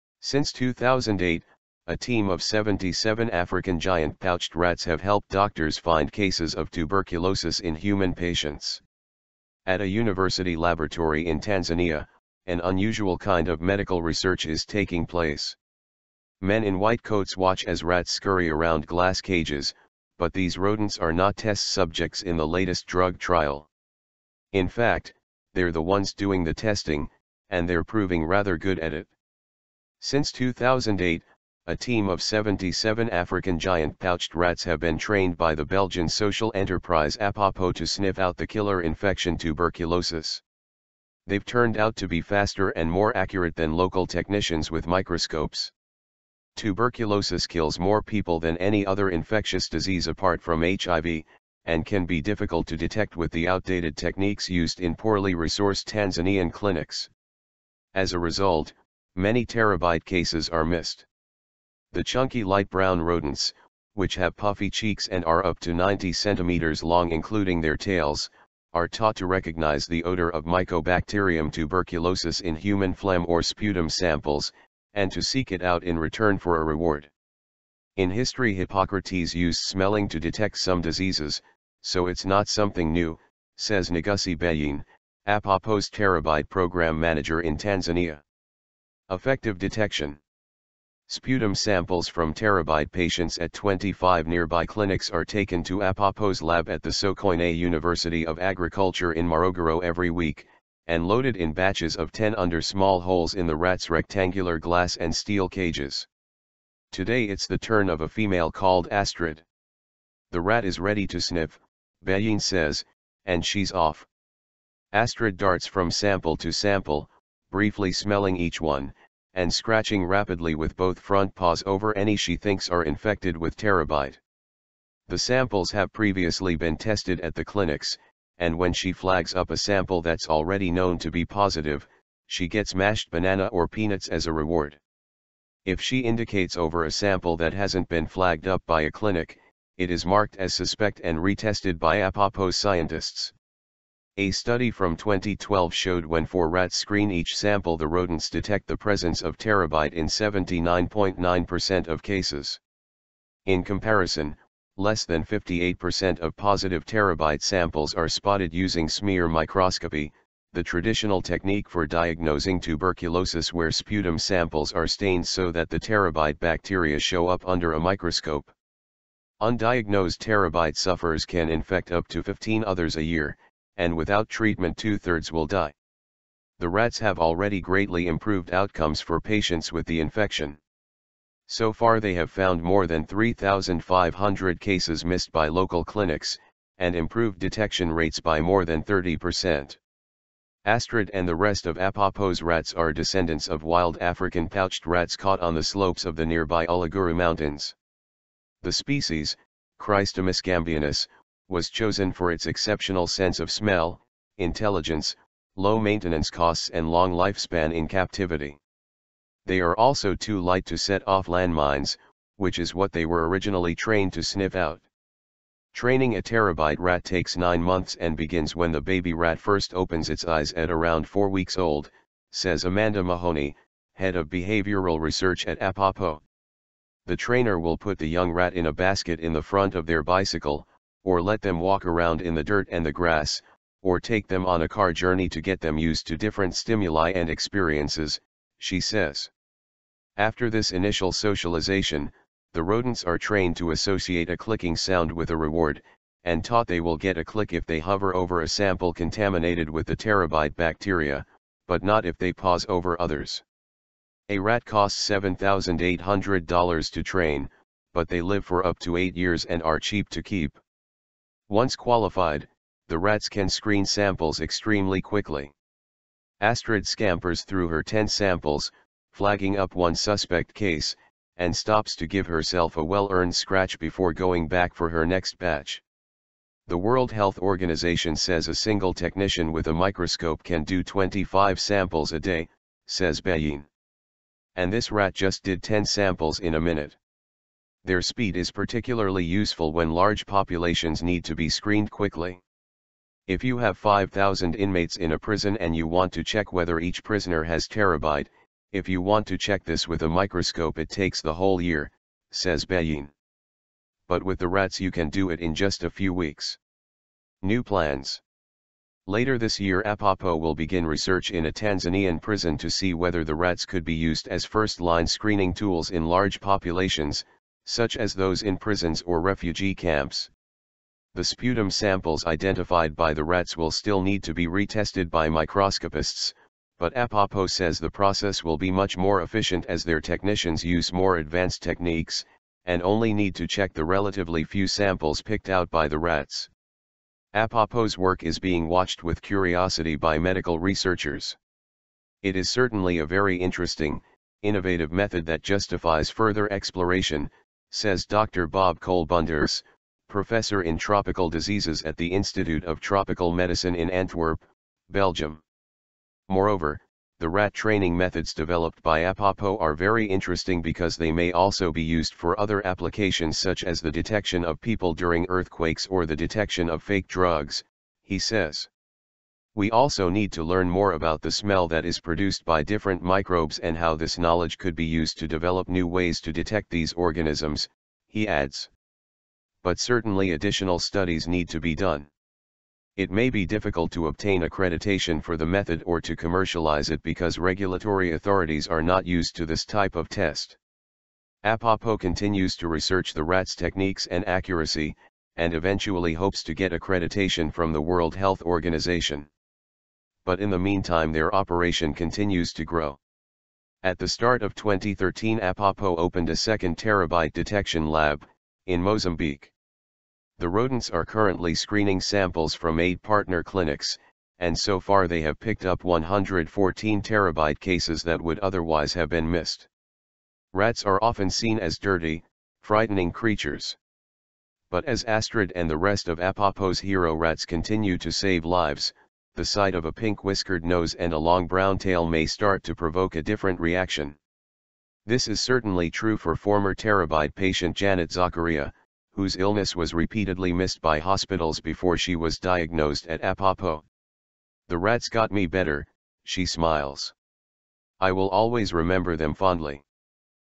Rats Can Sniff Out Tuberculosis In Tanzania
Since 2008, a team of 77 African giant pouched rats have helped doctors find cases of tuberculosis in human patients.

At a university laboratory in Tanzania, an unusual kind of medical research is taking place. Men in white coats watch as rats scurry around glass cages, but these rodents are not test subjects in the latest drug trial. In fact, they're the ones doing the testing, and they're proving rather good at it.

Since 2008, a team of 77 African giant pouched rats have been trained by the Belgian social enterprise Apopo to sniff out the killer infection tuberculosis. They've turned out to be faster and more accurate than local technicians with microscopes.

Tuberculosis kills more people than any other infectious disease apart from HIV, and can be difficult to detect with the outdated techniques used in poorly resourced Tanzanian clinics. As a result, many TB cases are missed.

The chunky light brown rodents, which have puffy cheeks and are up to 90 centimetres long including their tails, are taught to recognise the odour of mycobacterium tuberculosis in human phlegm or "sputum" samples, and to seek it out in return for a reward.

"In history Hippocrates used smelling to detect some diseases, so it's not something new," says Negussie Beyene, Apopo's TB programme manager in Tanzania.

Effective detection

Sputum samples from TB patients at 25 nearby clinics are taken to Apopo's lab at the Sokoine University of Agriculture in Morogoro every week, and loaded in batches of 10 under small holes in the rats' rectangular glass and steel cages. Today it's the turn of a female called Astrid. "The rat is ready to sniff," Beyene says, and she's off.

Astrid darts from sample to sample, briefly smelling each one, and scratching rapidly with both front paws over any she thinks are infected with TB. The samples have previously been tested at the clinics, and when she flags up a sample that's already known to be positive, she gets mashed banana or peanuts as a reward. If she indicates over a sample that hasn't been flagged up by a clinic, it is marked as "suspect" and re-tested by Apopo's scientists.

A study from 2012 showed when four rats screen each sample the rodents detect the presence of TB in 79.9 percent of cases.

In comparison, less than 58 percent of positive TB samples are spotted using smear microscopy, the traditional technique for diagnosing tuberculosis where sputum samples are stained so that the TB bacteria show up under a microscope. Undiagnosed TB sufferers can infect up to 15 others a year, and without treatment two-thirds will die.

The rats have already greatly improved outcomes for patients with the infection. So far they have found more than 3,500 cases missed by local clinics, and improved detection rates by more than 30 percent.

Astrid and the rest of Apopo's rats are descendents of wild African pouched rats caught on the slopes of the nearby Uluguru Mountains. The species, cricetomys gambianus, was chosen for its exceptional sense of smell, intelligence, low maintenance costs and long lifespan in captivity. They are also too light to set off landmines, which is what they were originally trained to sniff out.

Training a TB rat takes nine months and begins when the baby rat first opens its eyes at around four-weeks old, says Amanda Mahoney, head of behavioural research at Apopo.

"The trainer will put the young rat in a basket in the front of their bicycle, or let them walk around in the dirt and the grass, or take them on a car journey to get them used to different stimuli and experiences," she says.

After this initial socialisation, the rodents are trained to associate a clicking sound with a reward, and taught they will get a click if they hover over a sample contaminated with the TB bacteria, but not if they pause over others. A rat costs $7,800 to train, but they live for up to eight years and are cheap to keep.

Once qualified, the rats can screen samples extremely quickly. Astrid scampers through her 10 samples, flagging up one suspect case, and stops to give herself a well-earned scratch before going back for her next batch.

"The World Health Organisation says a single technician [with a microscope] can do 25 samples a day," says Beyene. "And this rat just did 10 samples in a minute."

Their speed is particularly useful when large populations need to be screened quickly. "If you have 5,000 inmates in a prison and you want to check whether each prisoner has TB - if you want to check this with a microscope it takes the whole year," says Beyene. "But with the rats you can do it in just a few weeks."